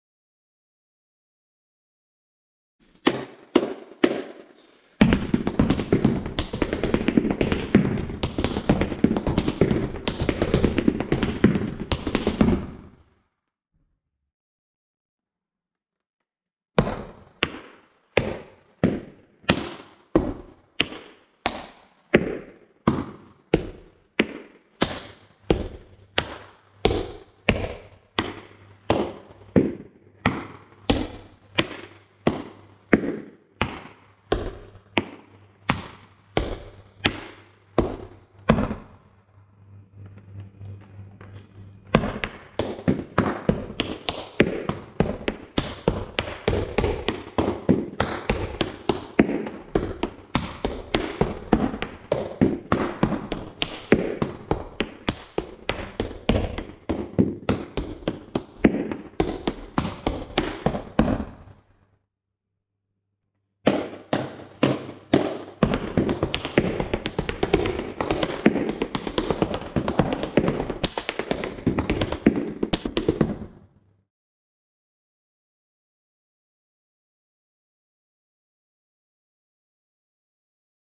Tap Dance - Studio Time ep.1 - The Tap Dance Teacher
Apart from my professionally recorded lessons, it's fun to teach with no words and share what I find in the studio. - Stealing steps is a big part of the art form!